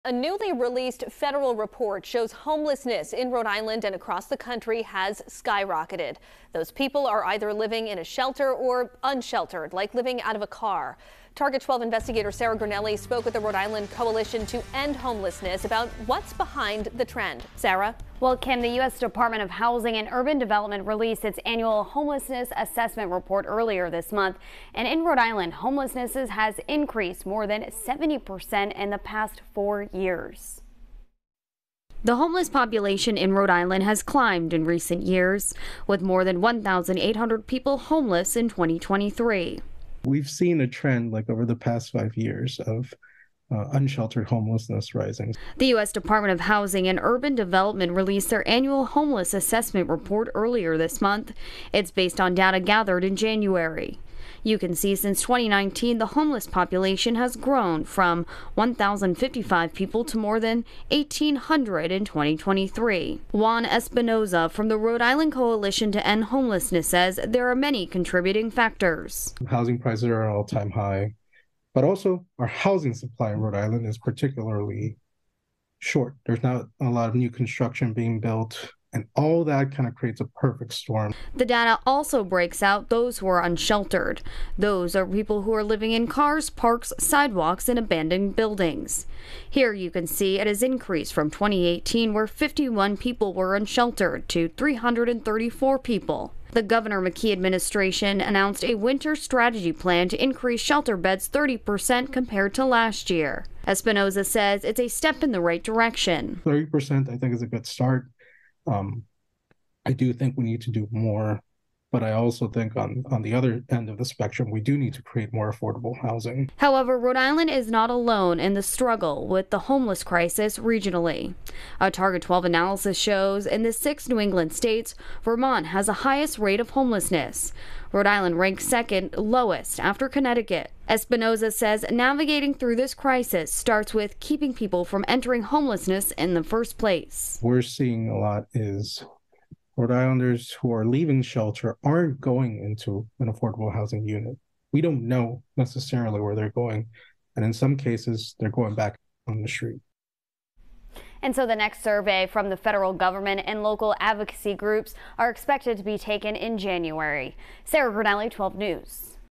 Homelessness in Rhode Island soared 70% over the past four years
According to newly released federal data, more than 1,800 people in Rhode Island were experiencing homelessness in early 2023.